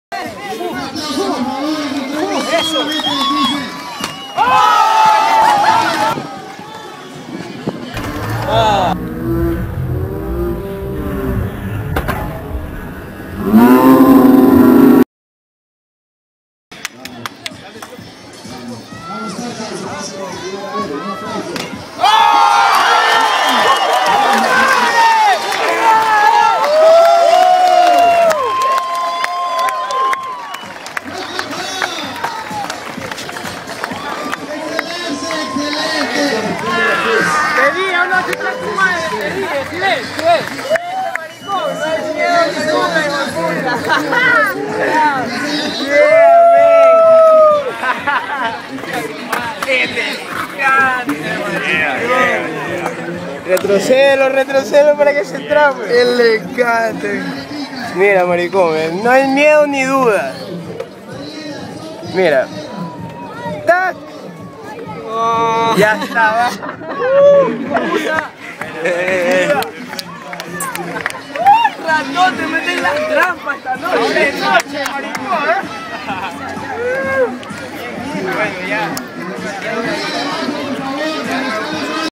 Mejor truco playas Ecuador
360 top soul
Elegido y premiado como el mejor truco en sudamericano playas Ecuador agosto 2012